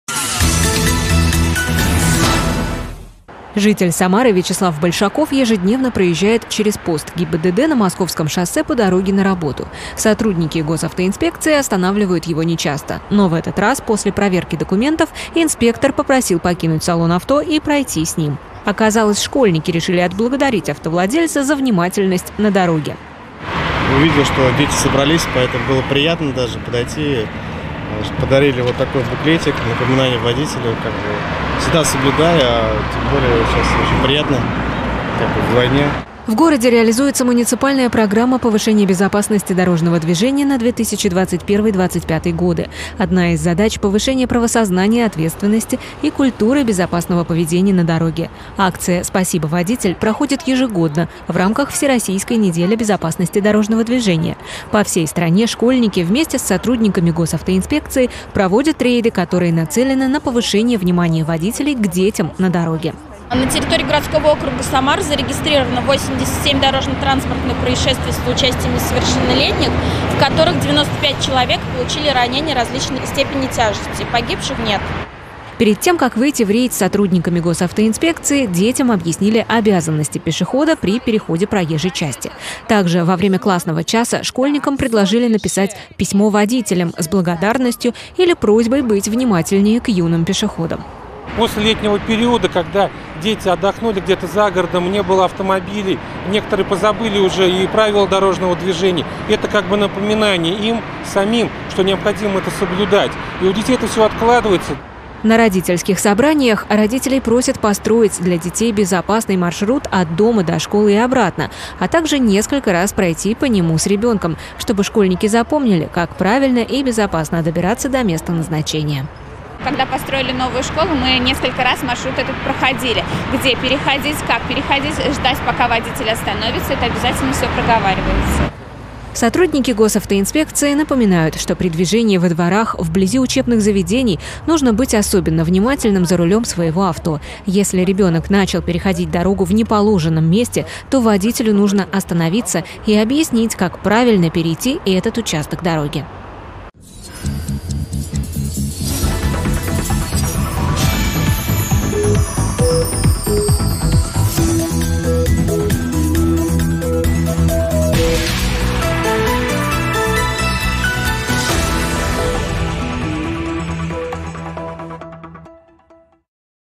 Специальный репортаж от 08.10.2021. "Акция "Спасибо, водитель"
В городе реализуется муниципальная программа "Повышение безопасности дорожного движения" на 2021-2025 годы. Одна из задач - повышение правосознания, ответственности и культуры безопасного поведения на дороге. Акция "Спасибо, водитель" проходит ежегодно, в рамках всероссийской недели безопасности дорожного движения. По всей стране школьники вместе с сотрудниками Госавтоинспекции проводят рейды, которые нацелены на повышение внимания водителей к детям на дороге.